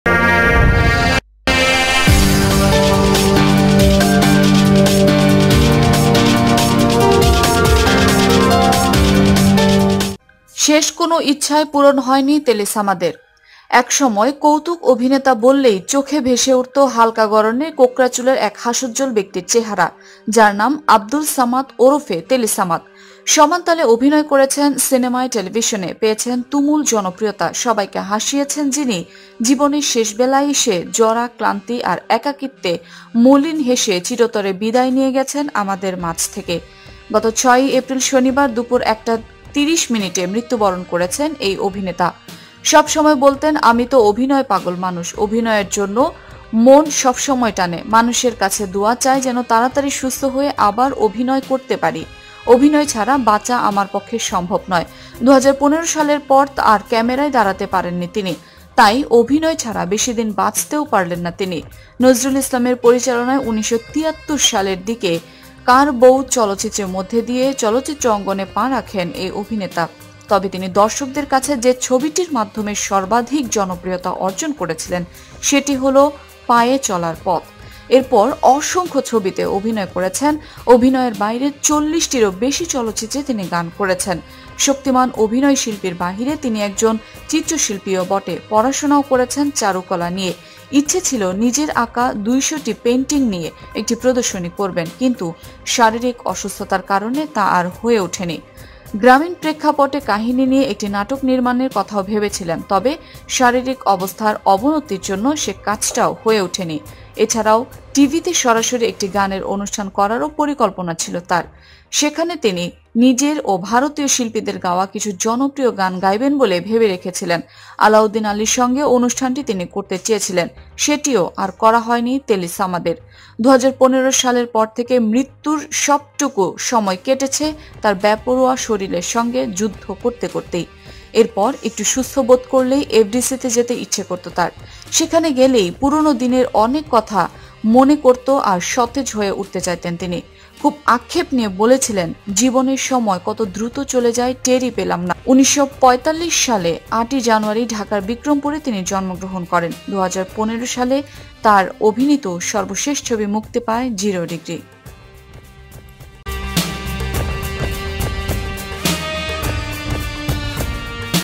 শেষ কোনো ইচ্ছাই পূরণ হলোনা টেলি সামাদের ।। Teli Samad Death
শেষ কোনো ইচ্ছাই পূরণ হলোনা টেলি সামাদের ।।

একসময় কৌতুক অভিনেতা বললেই চোখে ভেসে উঠতো হালকা গড়নের, কোঁকড়া চুলের এক হাস্যজ্জ্বল ব্যক্তির চেহারা। তার নাম আব্দুস সামাদ ওরফে টেলি সামাদ। সমানতালে অভিনয় করেছেন সিনেমায়, টেলিভিশনে। পেয়েছেন তুমুল জনপ্রিয়তা। সবাইকে হাসিয়েছেন যিনি, জীবনের শেষ বেলায় এসে জরা- ক্লান্তি আর একাকীত্বের মলিন হাঁসি হেসে চিরতরে বিদায় নিয়ে গেলেন আমাদের মাঝ থেকে।
৬ এপ্রিল, শনিবার দুপুর ১টা ৩০ মিনিটে মৃত্যুবরণ করেছেন এই অভিনেতা।
সবসময় বলতেন, আমিতো অভিনয় পাগল মানুষ। অভিনয়ের জন্য মন সবসময় টানে। মানুষের কাছে দোয়া চাই যেন তাড়াতাড়ি সুস্থ হয়ে আবার অভিনয়ে ফিরতে পারি। অভিনয় ছাড়া বাঁচা আমার পক্ষে অসম্ভব।
২০১৫ সালের পর আর ক্যামেরার সামনে দাঁড়াতে পারেননি। আর তাই অভিনয় ছাড়া বেশি দিন বাঁচতেও পারলেন না তিনি। নজরুল ইসলামের পরিচালনায় ১৯৭৩ সালের দিকে ‘কার বৌ’ চলচ্চিত্রের মধ্য দিয়ে এই অঙ্গনে পা রাখেন এই অভিনেতা। তবে তিনি দর্শকদের কাছে যে ছবিটির মাধ্যমে সর্বাধিক জনপ্রিয়তা অর্জন করেন সেটি হলো ‘পায়ে চলার পথ’।
এরপর অসংখ্য ছবিতে অভিনয় করেছেন। অভিনয়ের বাইরে ৪০টির বেশি চলচ্চিত্রে তিনি গানও গেয়েছেন।
শক্তিমান অভিনয় শিল্পীর বাহিরে তিনি একজন চিত্রশিল্পীও বটে, পড়াশোনাও করেছেন চারুকলা নিয়ে। ইচ্ছা ছিলো নিজের আঁকা ২০০ পেইন্টিং নিয়ে একটা প্রদর্শনী করবেন। কিন্তু শারীরিক অসুস্থতার কারণে আর করা হয়ে উঠেনি।
গ্রামীণ প্রেক্ষাপটের কাহিনী নিয়ে একটি নাটক নির্মাণের কথাও ভেবেছিলাম। তবে শারীরিক অবস্থার অবনতির জন্য আর কাজটা করা হয়ে ওঠেনি।
এছাড়া টিভিতে সরাসরি একটি গানের অনুষ্ঠান করারও পরিকল্পনা ছিলো তার। যেখানে তিনি নিজের ও ভারতের শিল্পীদের গাওয়া কিছু জনপ্রিয় গান গাইবেন বলে ভেবে রেখেছিলেন। আলাউদ্দিন আলীর সঙ্গে অনুষ্ঠানটি তিনি করতে চেয়েছিলেন। সেটিও আর করা হলো না টেলি সামাদের।
২০১৫ সাল থেকে মৃত্যুর সময়টুকু, কেটে গেল তার বেপরোয়া শরীরের সঙ্গে যুদ্ধ যুদ্ধ করতে করতেই। এরপরও একটু সুস্থ বোধ করলেই এফডিসিতে যেতে ইচ্ছে করতো তার। সেখানে গেলেই পুরানো দিনের অনেক কথা মনে করে আরও সতেজ হয়ে উঠতে চাইতেন। খুব আক্ষেপ নিয়ে বলেছিলেন, জীবনের সময় কত দ্রুত চলে গেল টেরই পেলাম না।
১৯৪৫ সালের ৮ জানুয়ারি ঢাকার বিক্রমপুরে তিনি জন্মগ্রহণ টেলি সামাদ। ২০১৫ সালে তার অভিনীত সর্বশেষ ছবি মুক্তি পায় ‘জিরো ডিগ্রী’।